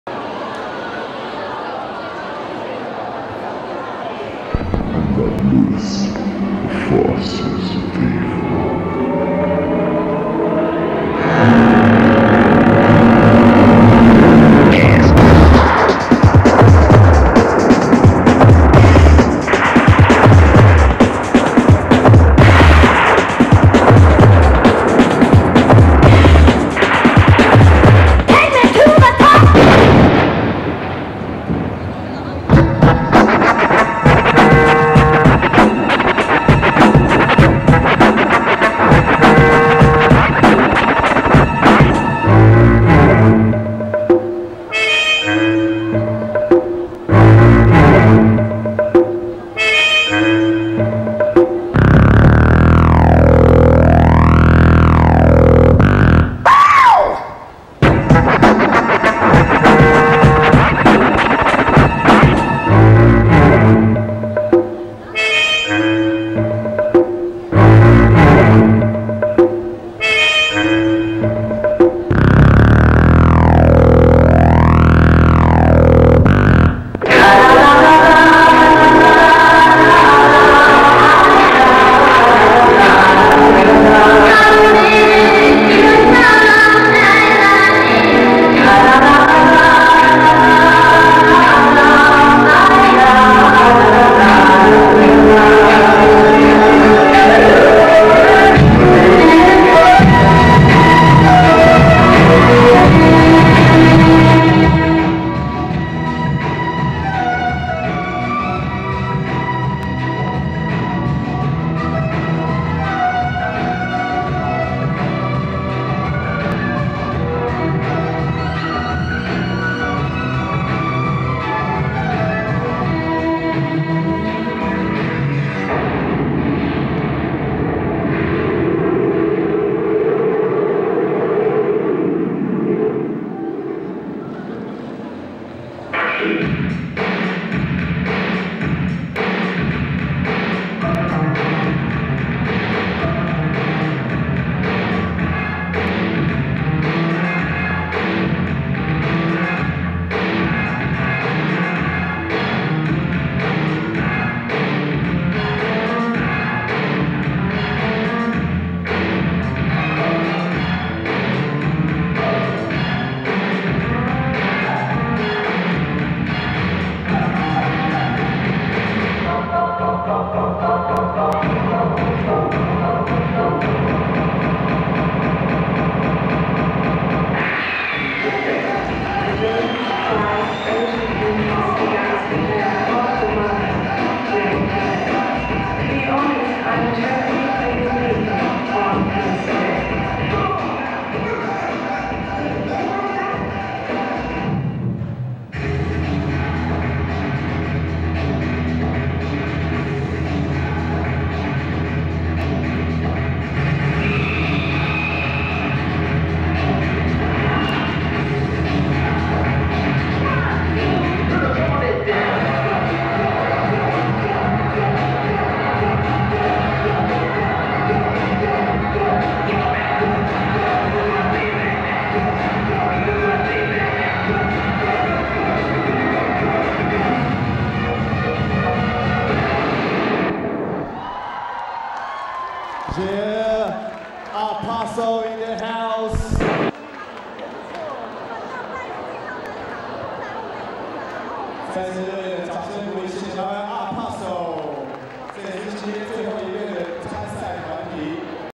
17 APASO｜20000429 NIKE HOT創意街舞總決賽（官方版）
20000429 NIKE HOT創意街舞總決賽（官方版）
不是我拍攝的，沒記錯應該是2001參與HIP-HOOP工作團隊時、工作人員給我的前一年錄影帶。
我只負責把它轉檔上YOUTUBE做紀錄。